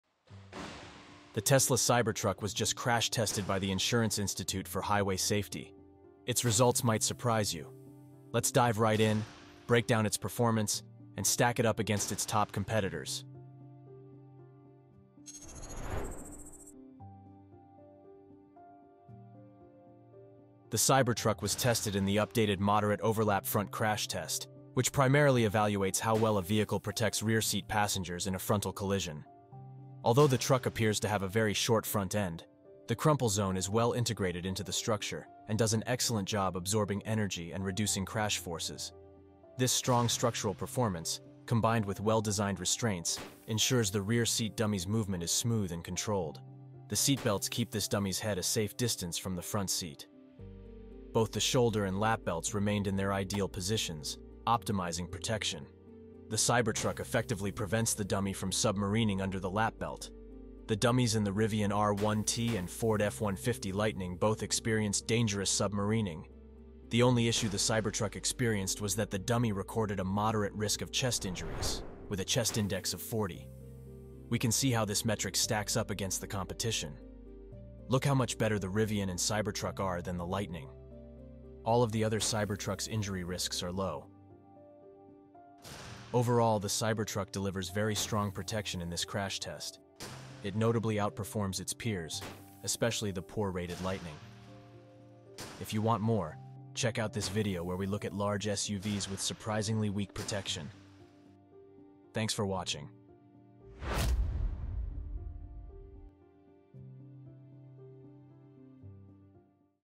Cybertruck Aces Crash Test
An analysis of the Tesla Cybertruck's performance in the updated moderate overlap crash test, conducted by the Insurance Institute for Highway Safety. We'll also compare the Cybertruck's results to the Rivian R1T and Ford F-150 Lightning.

Video footage courtesy of the IIHS and Tesla. Editing by Crash Core.